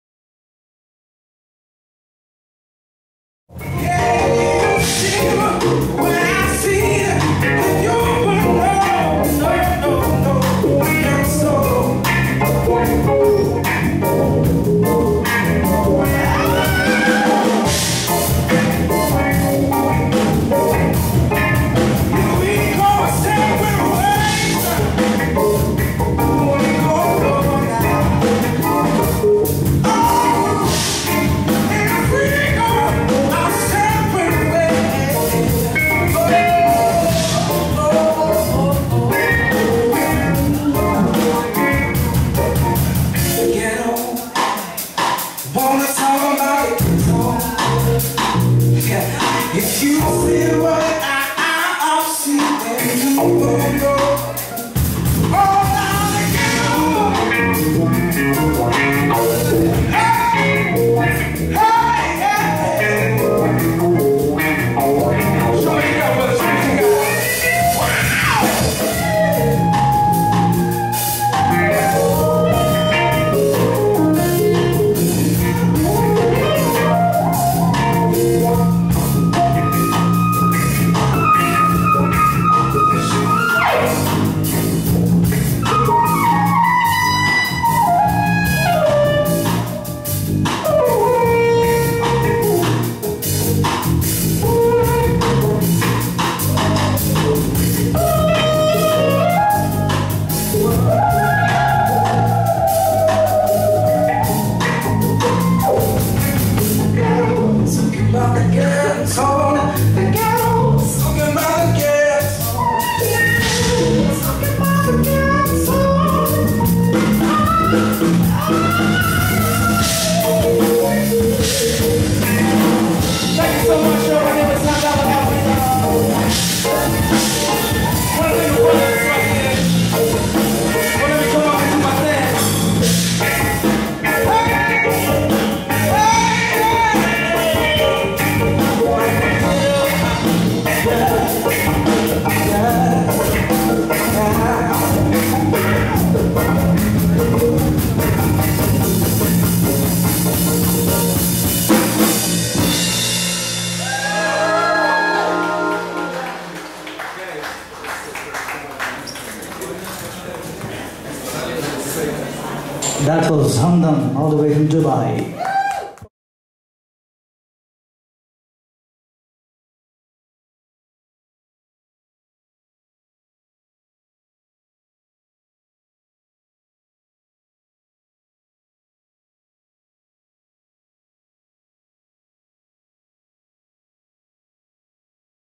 Smoking Chutney featuring Hamdan Al-Abri
Thursday Night Live at the Sofala, Nerul, Goa.